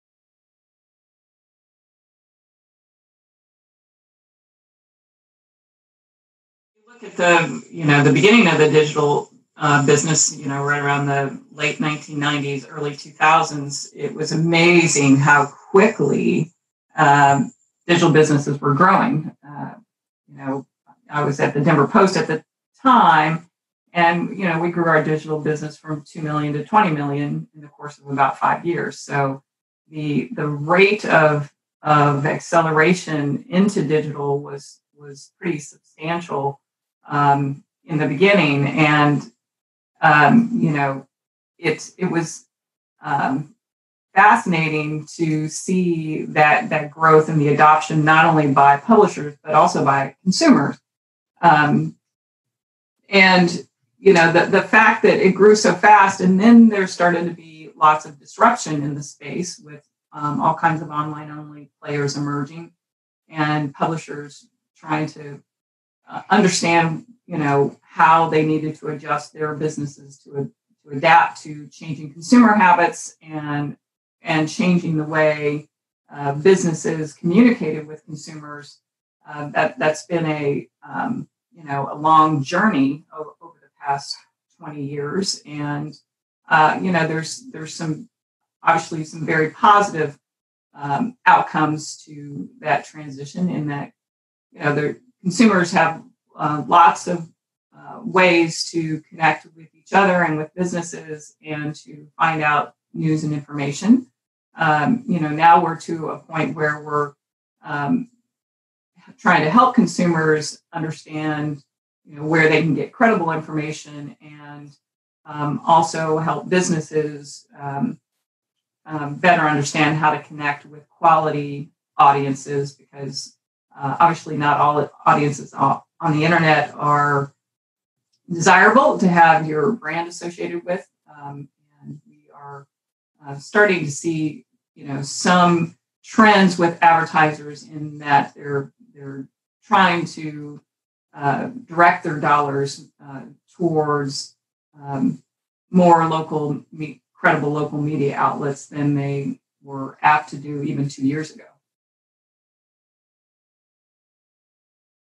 Fran Wills, Local Media Consortium — What Was the Most Eye-Opening Shift From Print to Digital?
TL;DR
There are so many players that the focus has transitioned from finding new audiences to ensuring the mass of audiences have credible sources to rely on.

TS;WM
“Well, if you look at the beginning of the digital business in the late 1990s - early 2000s, it was amazing how quickly digital businesses were growing.

I was at The Denver Post at the time and we grew our digital business from $2 million to $20 million in the course of about five years, so the rate of acceleration into digital was pretty substantial in the beginning. And, it was fascinating to see that growth and the adoption, not only by publishers, but also by consumers. And the fact that it grew so fast and then there started to be lots of disruption in the space with all kinds of online-only players emerging, and publishers trying to understand how they needed to adjust their businesses to adapt to changing consumer habits and changing the way businesses communicated with consumers — that’s been a long journey over the past 20 years.

And there’s actually some very positive outcomes to that transition in that consumers have lots of ways to connect with each other and with businesses, and to find out news and information. Now, we’re to a point where we’re trying to help consumers understand where they can get credible information. And also, help businesses better understand how to connect with quality audiences because, actually, not all audiences on the internet are desirable to have your brand associated with. We are starting to see some trends with advertisers in that they are trying to direct their dollars towards more credible local media outlets than they were apt to do even two years ago.”